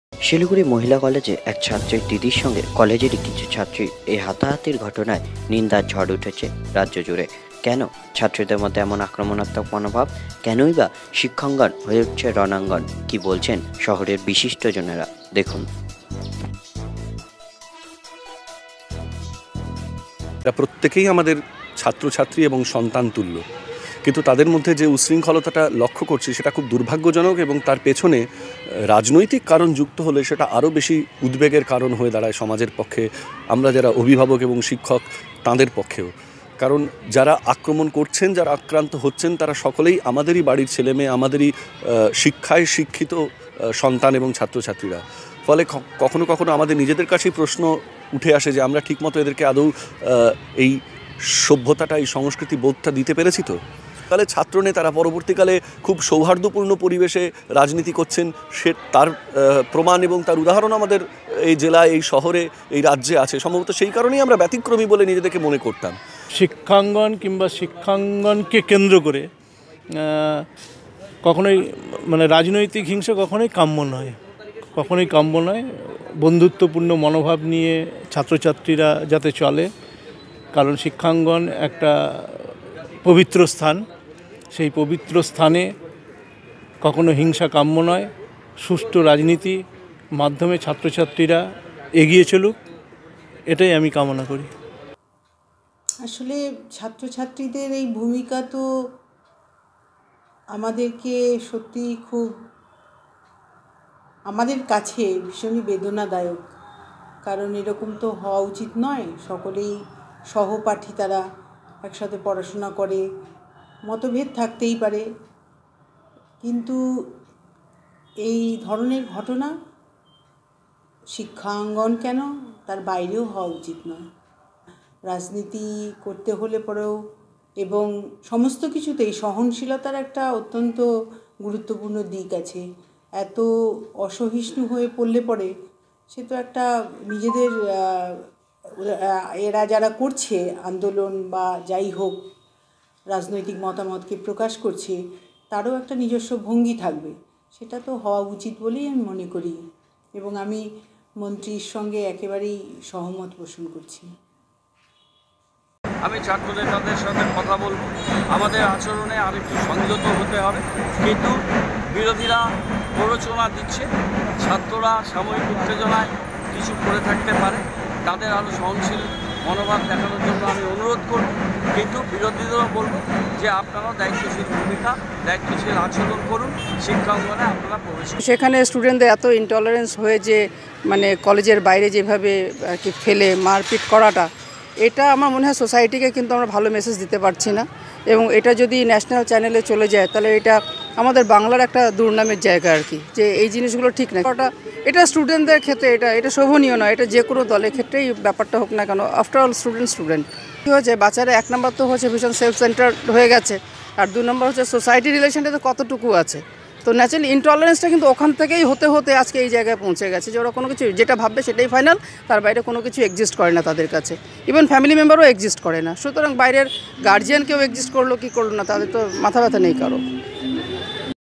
মহিলা কলেজের ঘটনায় কী বলছেন শিলিগুড়ির বিশিষ্ট মানুষেরা?
শিক্ষাঙ্গণ কেন রণাঙ্গণ?‌

কেন এমন আচরণ ছাত্রীদের?‌

মহিলা কলেজের ঘটনায় কী বলছেন শহরের বিশিষ্ট মানুষেরা?